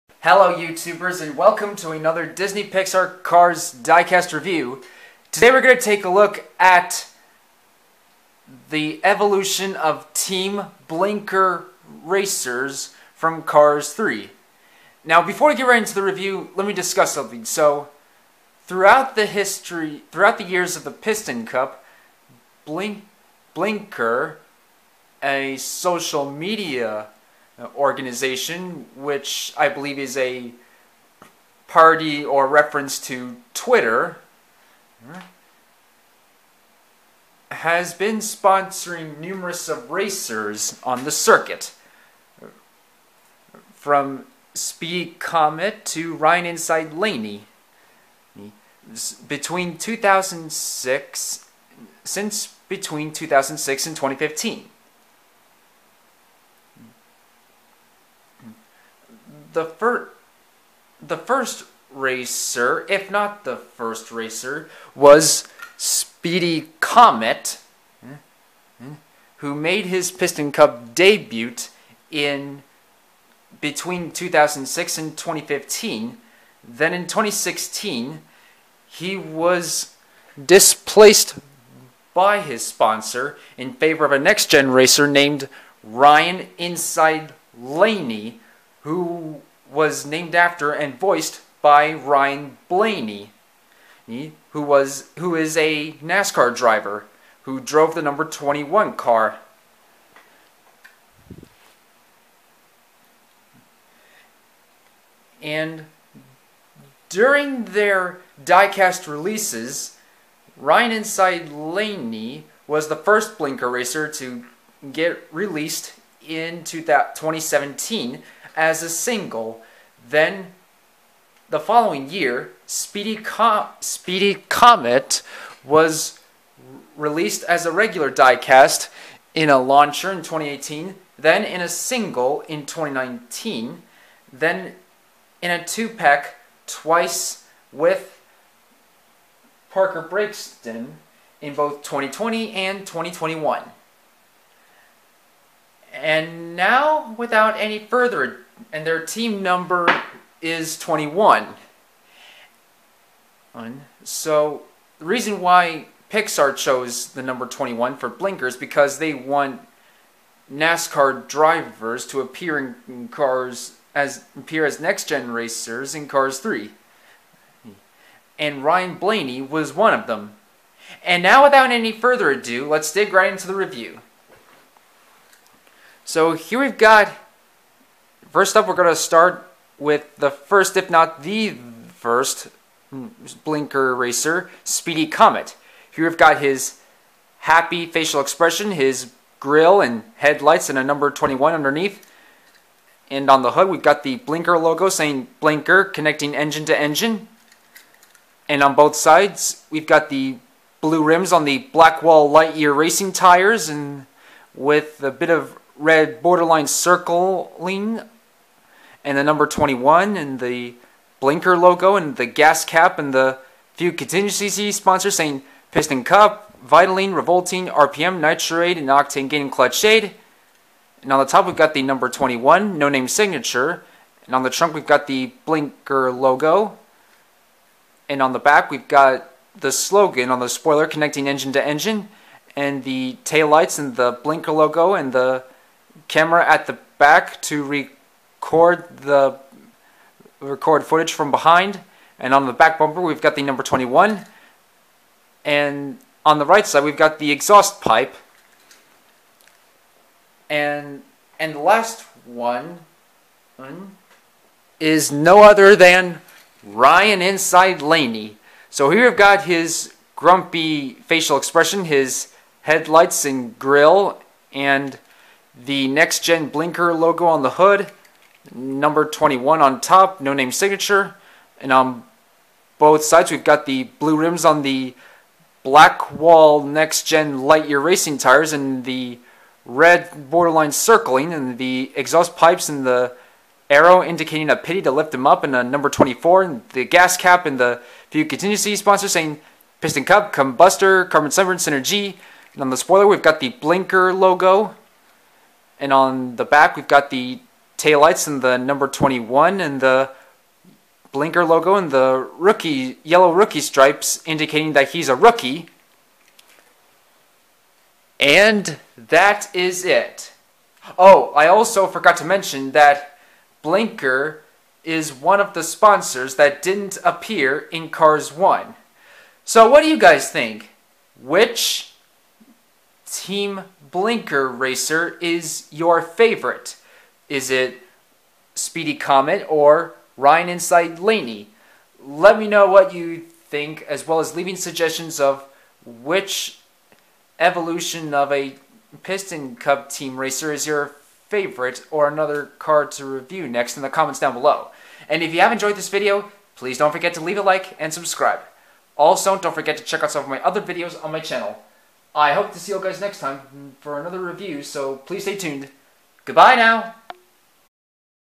Disney Pixar Cars 3 Team Blinkr Piston cup racers (Speedy & Ryan) diecasts
Hello everybody & today, we're gonna take a look at the Evolution of Team Blinkr Piston cup racers (Speedy Comet & Ryan Inside Laney).